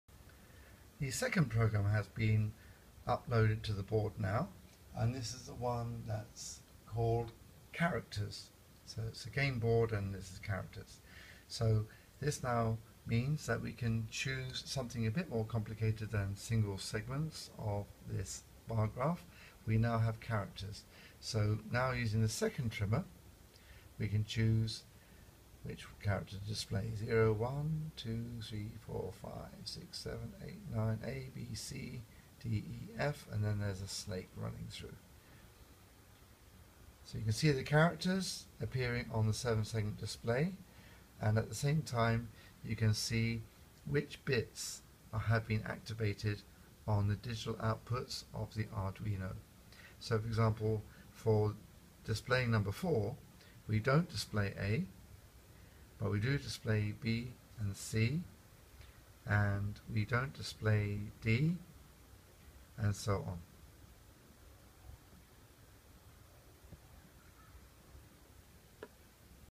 IE2 2 Characters
Arduino sketch demonstration using "Arduino Programming Facilitator" board custom made at Singapore Polytechnic